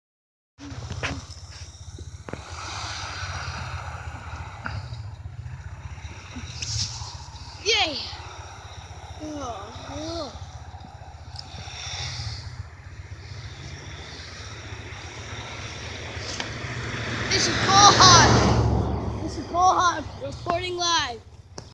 SnowDay R&R 1
Casey wanted a snow day. Rachel doin Ice Donuts!